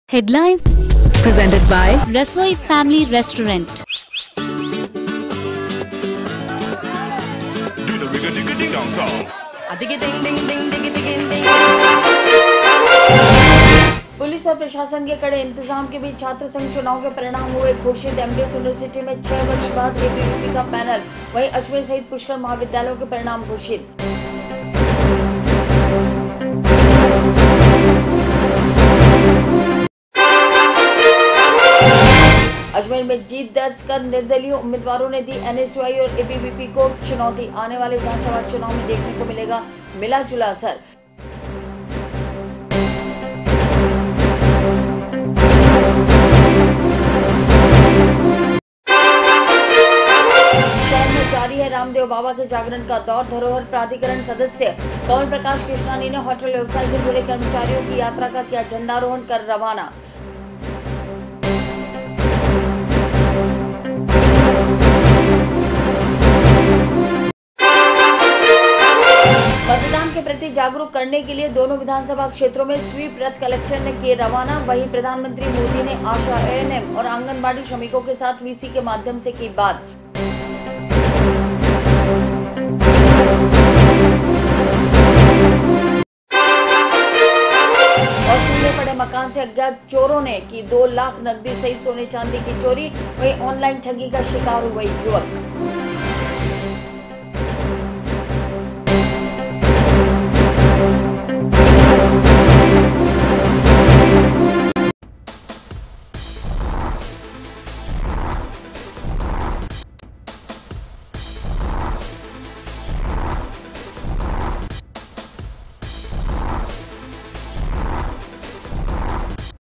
Today's Headlines 11 SEP 2018
पुलिस और प्रशासन के कड़े इंतजाम के बीच छात्रसंघ चुनाव के परिणाम हुए घोषित। एमडीएस युनिवर्सिटी में 6 वर्ष बाद एबीवीपी का पैनल। वहीं अजमेर सहित पुष्कर महाविद्यालयों के परिणाम घोषित।

अजमेर में जीत दर्ज करवा कर निर्दलीय उम्मीदवारों ने दी एनएसयूआई और एबीवीपी को दी चुनौती। आने वाले विधानसभा चुनाव में देखने को मिलेगा मिलाजुला असर।

शहर में जारी है रामदेव बाबा के जागरण का दौर। धरोहर प्राधिकरण सदस्य कंवल प्रकाश किशनानी ने होटल व्यवसाय से जुड़े कर्मचारियों की यात्रा का किया झंडारोहण कर रवाना।

मतदान के प्रति जागरूक करने के लिए के दोनों विधानसभा क्षेत्रों में स्वीप रथ कलेक्टर ने किये रवाना। वहीं प्रधानमंत्री मोदी ने आशा, एएनएम और आंगनबाड़ी श्रमिकों के साथ वीसी के माध्यम से की बात।

और सूने पड़े मकान से अज्ञात चोरों ने की 2 लाख नगदी सहित सोने चांदी की चोरी। वहीं ऑनलाइन ठगी का शिकार हुए युवक।